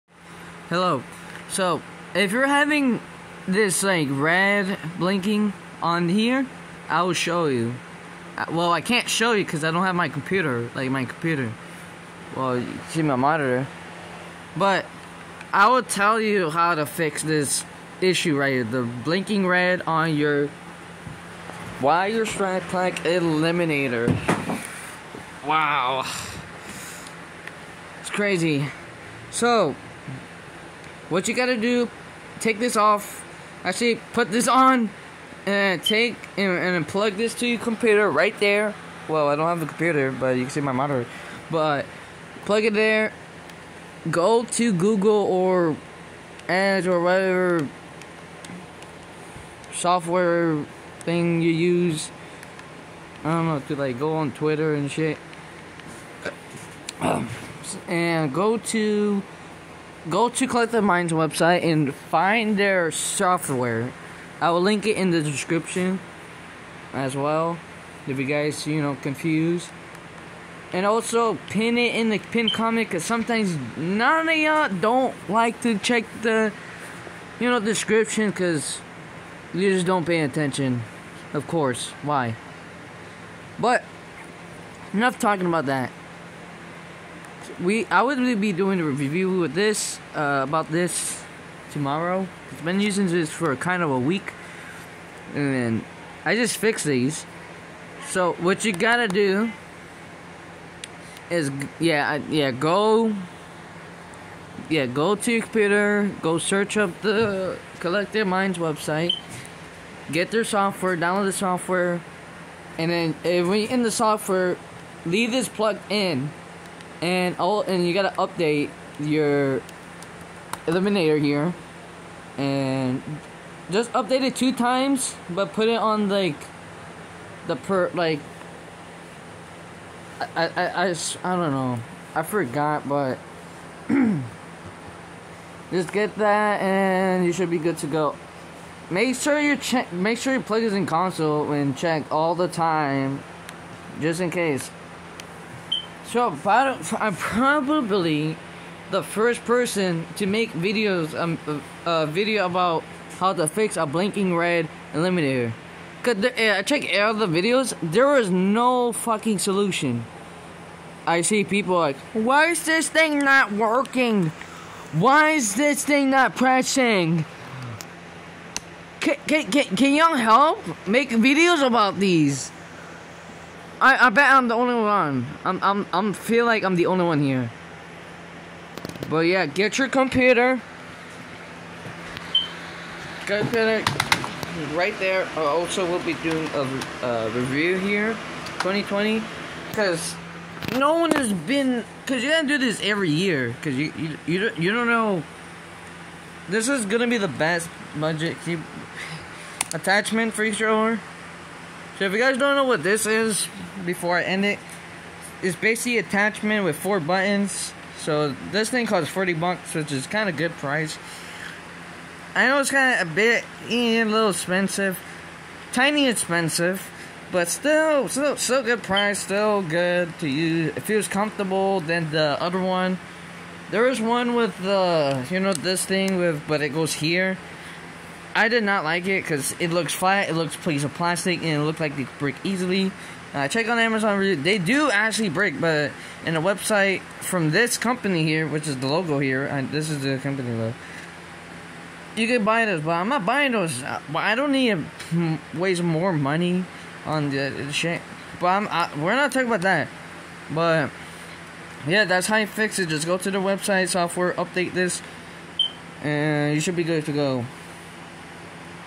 How to solve the blinking red eye problem on strike pack eliminator.
so this is a video how to solve the red blinking problem on the mod pack by the way I feel like I'm the first person to make a video about this issue because I checked on YouTube when I first started having this problem 6 days ago and there was no videos about that it was just other videos of people having the same problem. Here's the link to software and follow the steps to update it